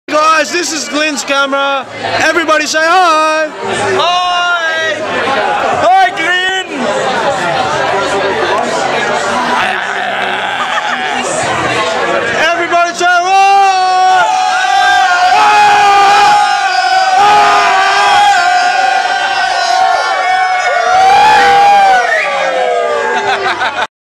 Waitangi Day, London 2007
A short video taken by Jason Bennett at the Stanhope Arms Hotel